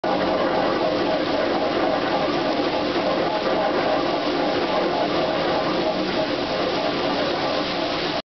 Hagley Museum - turbine
Water-powered turbine in operation